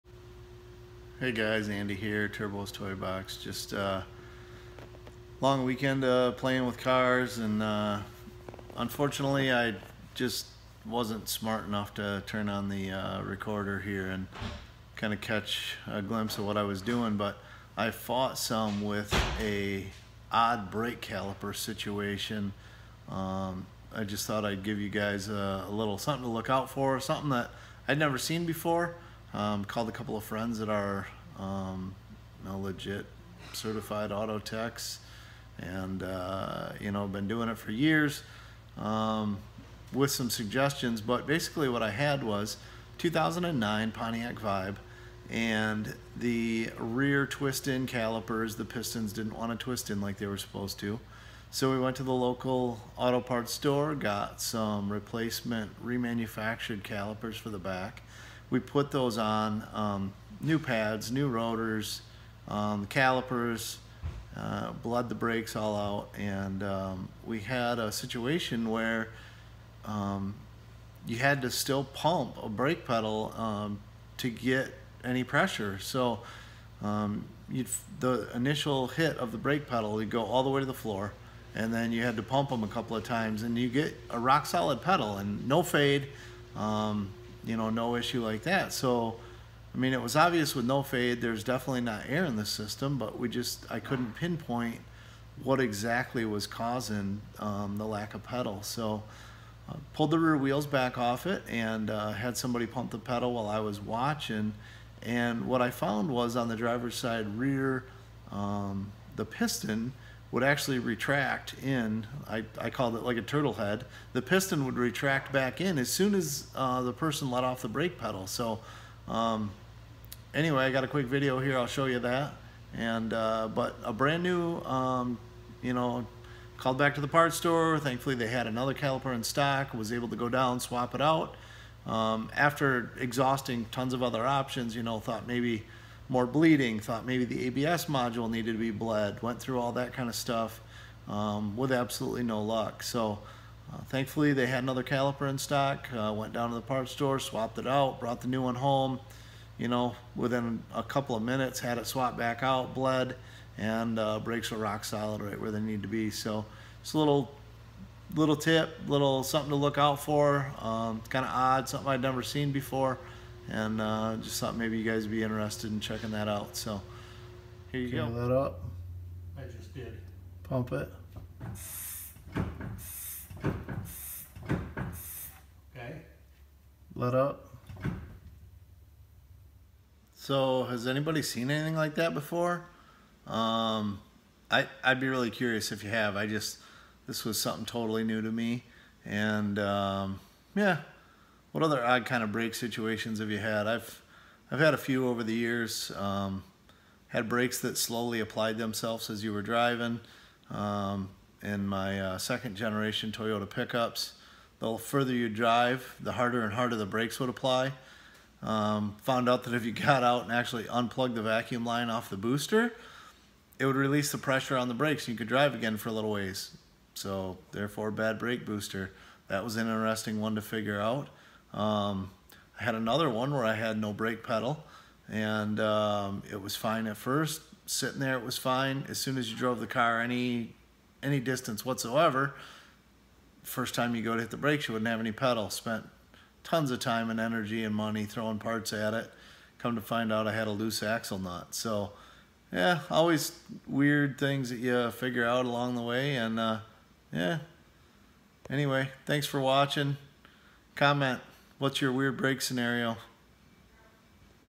Weekend head scratcher
Pontiac Vibe, Toyota Matrix brake issue. No pedal pressure after new calipers, pads and rotors. Properly bled, no air, no leaks... reman calipers got me